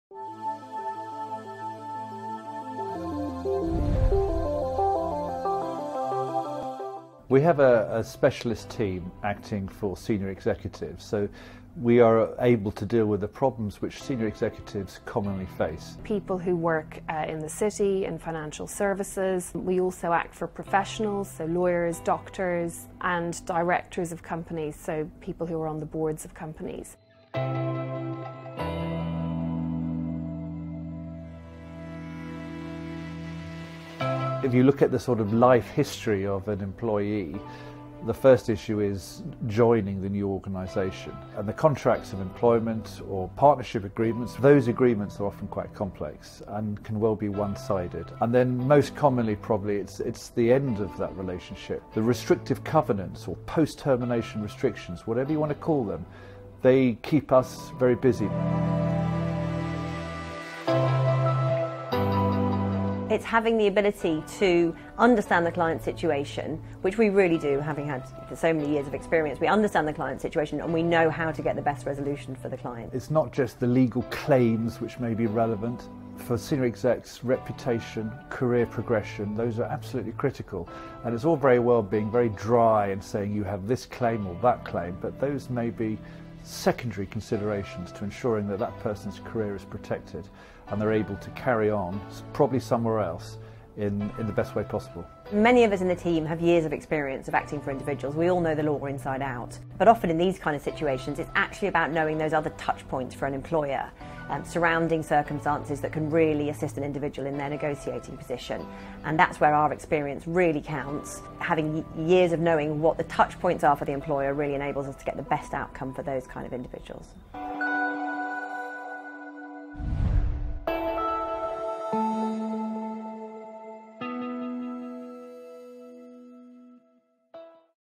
Employment Law Advice For Senior Executives
Having experienced and specialist legal advisers for your employment law issues is extremely important when you are a Senior Executive. Slater and Gordon's employment experts have a senior executive team who deal with these issues which arise and are well versed in advising individuals to put them in the best possible position. Having this team fighting on your side can make all the difference. This may be before they start a new job advising on contracts and terms or assisting in the event of any issues.

If you need an employment lawyer to protect your career or reputation call Slater and Gordon Lawyers on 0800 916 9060 or visit our